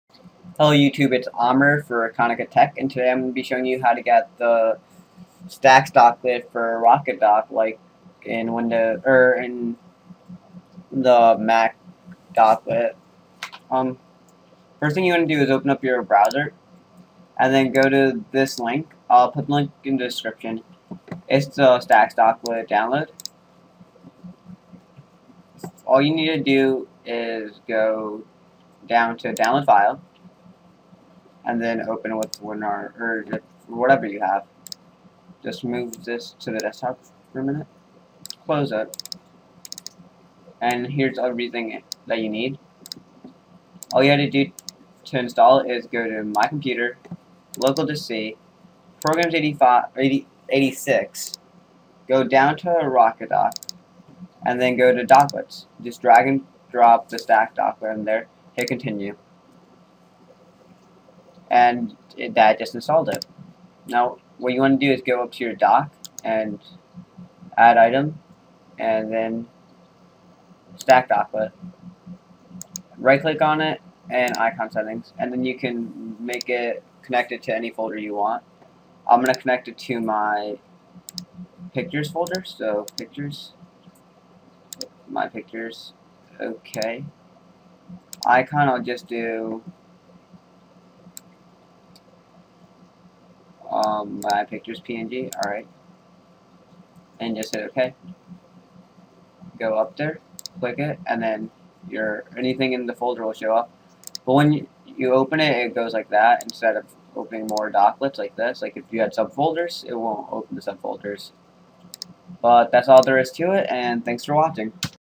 Windows Tutorial : How to install Stacks Docklet for Rocket Dock [Tut] - Easy to follow
Sorry for my voice, it's really annoying. Sorry for the uhs and ums, and small erros when naming stuff.
Anyways here's the tutorial , hope it's easy enough for you guys to follow.

Stacks gone after restart? Solution right here! :

Log in as admin to have the administrative rights.

1. Navigate to the RocketDock folder under program files
C/Program Files/RocketDock.

2. Right click on the RocketDock folder, and select properties, click the security tab at the top.

3. You should see a list of groups and user names of your pc, click edit.

4. Click on your user name..should be called "Users(your name)"

5. Check the box underneath to give yourself "Full Control", click Ok.

6. Done, your user account now has full permission to this folder.
To test it close the application or restart and you settings should be saved.